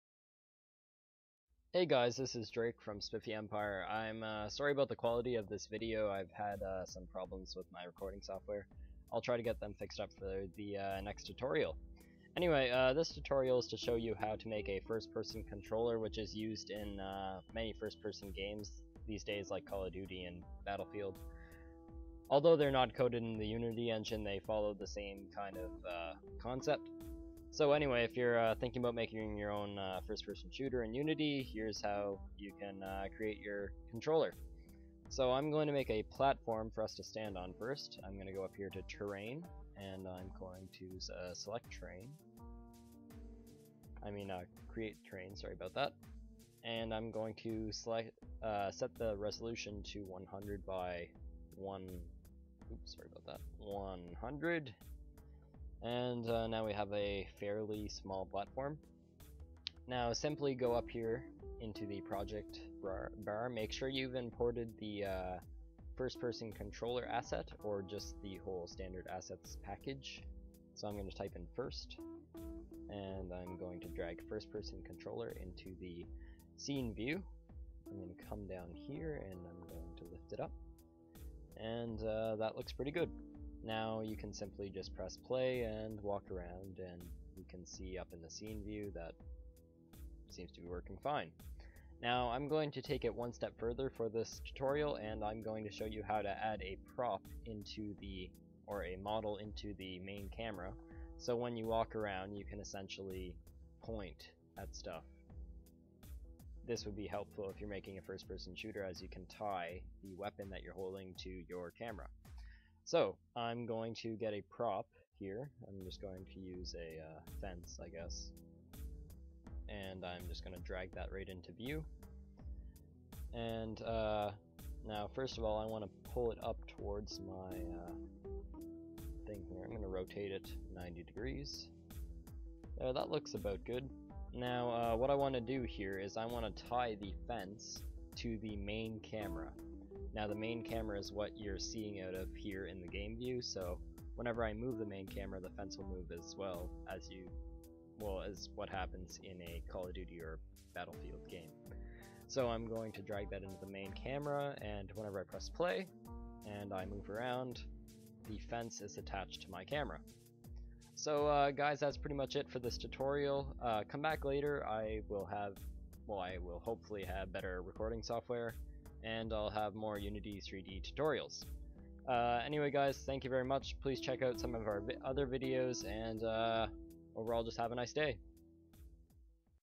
Unity3D Tutorial #1 - First Person Controllers
Today I show you how to add a first person controller to a Unity3D, and associate a model with it (adding a gun to a first person shooter).